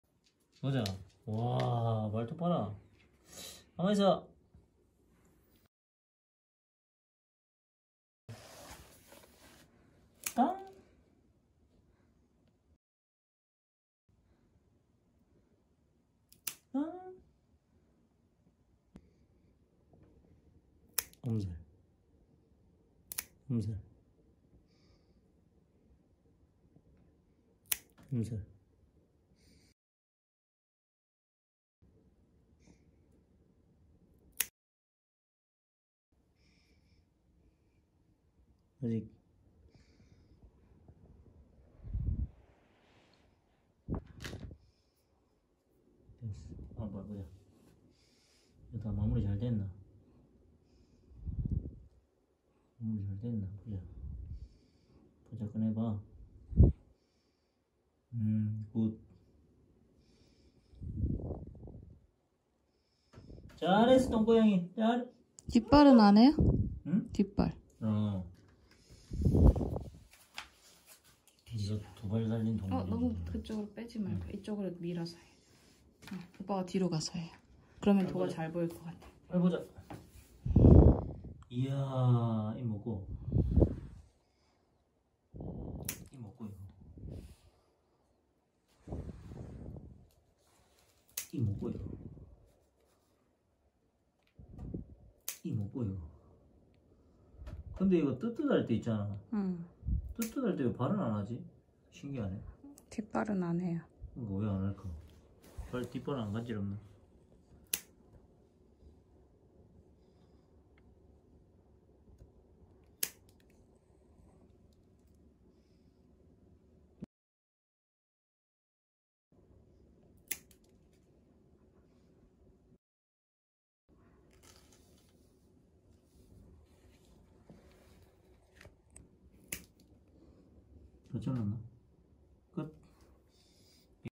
[쿤도브라더스] [웹툰] 77. 발톱
[ Today’s video ]
쿤도 브라더스 웹툰 77화
- 누나도 발톱 깎아주고 싶어 도야.. 그럼 너의 발바닥 젤리를 맘껏 만질 수 있겠지..?😗

[ 시바견 쿤이와 고양이 도의 일상 이야기 입니다 ]